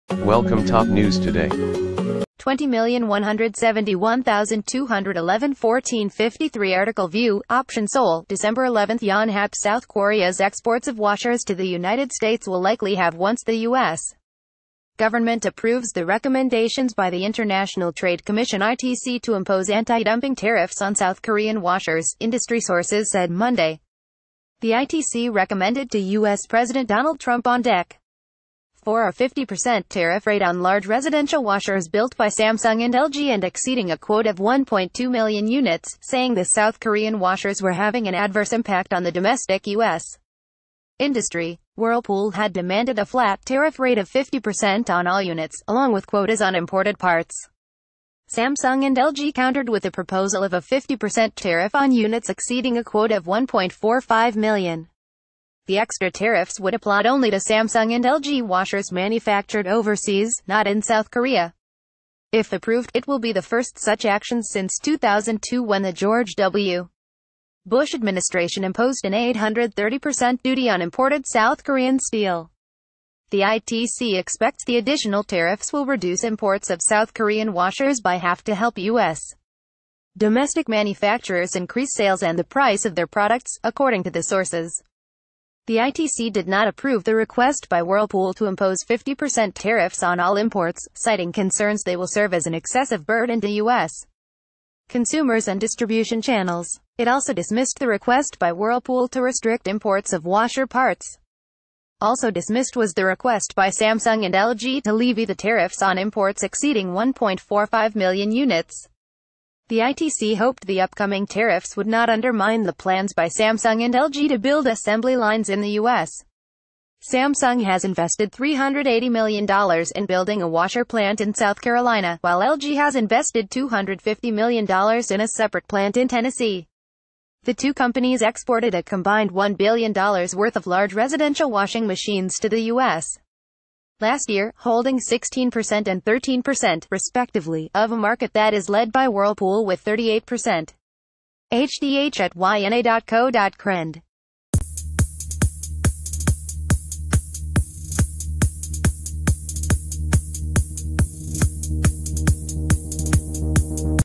BREAKING: Washing machine exports to the United States to cut in half if trump approved anti-dumpin
BREAKING: Washing machine exports to the United States to cut in half if trump approved anti-dumping tariffs: sources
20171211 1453 Article View Option SEOUL, Dec. 11 Yonhap South Koreas exports of washers to the United States will likely halve once the U.S. government approves the recommendations by the Internationa...